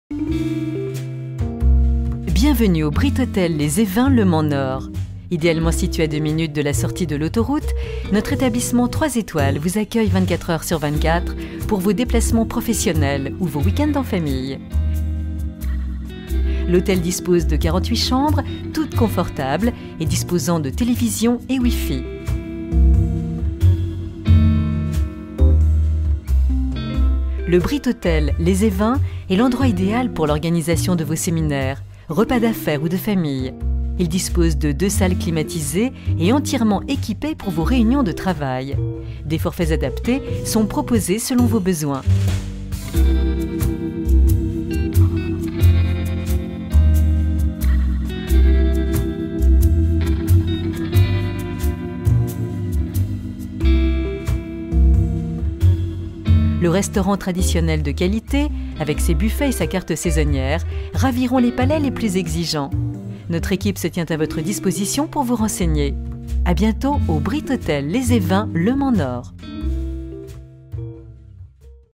Brit Hôtel Les Evens, hôtel et restaurant au Mans.
- Pour une escale ou un court séjour dans la ville du Mans, choisissez le confort et l'élégance en optant pour le Brit Hôtel Les Evens ! Situé sur l'axe Rennes-Paris au cœur de la campagne sarthoise et tout proche de l'autoroute A10, cet hôtel aux multiples équipements sera le lieu idéal pour votre séjour touristique ou d'affaires. Pour vous restaurer, n'hésitez pas à venir profiter de la cuisine traditionnelle du restaurant de l'hôtel, ouvert du lundi midi au vendredi midi et le week-end sur réservation.
Le Brit Hôtel Les Evens, ce sont aussi deux salles de séminaire climatisées pouvant accueillir de 25 à 50 personnes.